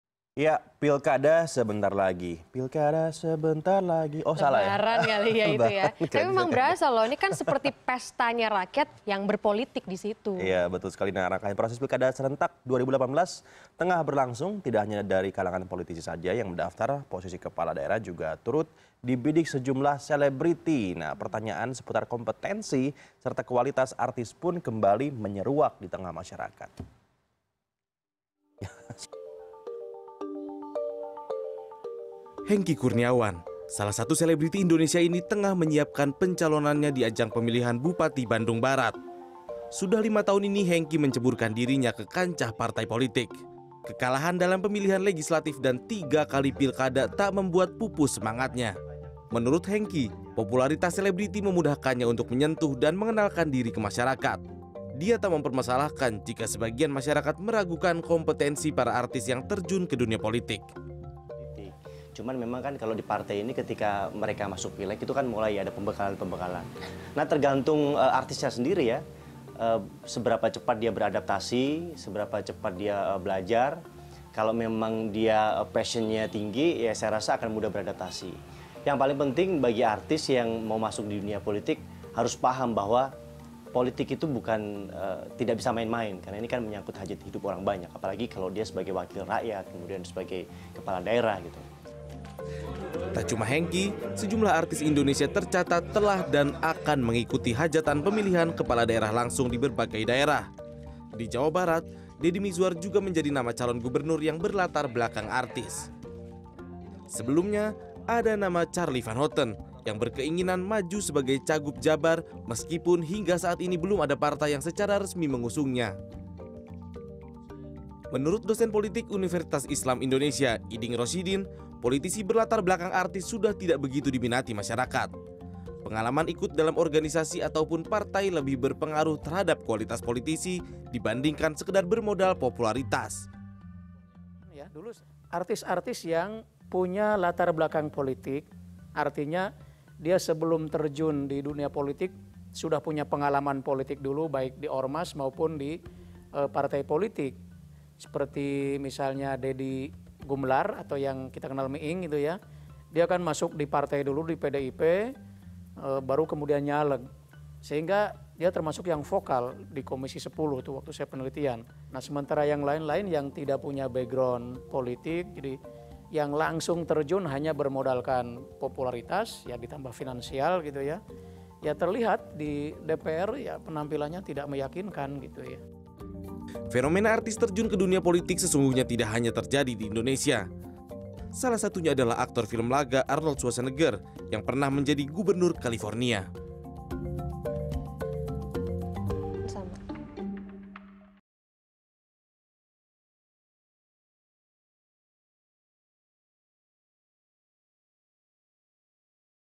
Geliat Artis Maju Pilkada
Rangkaian proses Pilkada serentak 2018 tengah berlangsung. Tidak hanya dari kalangan politisi saja yang mendaftar, posisi kepala daerah juga turut dibidik sejumlah selebritas.

Di tengah ramainya partisipasi artis dalam Pilkada, pertanyaan seputar kompetensi serta kualitas artis pun kembali menyeruak di tengah masyarakat

Like & Follow Facebook:
CNN Indonesia

cnnindonesia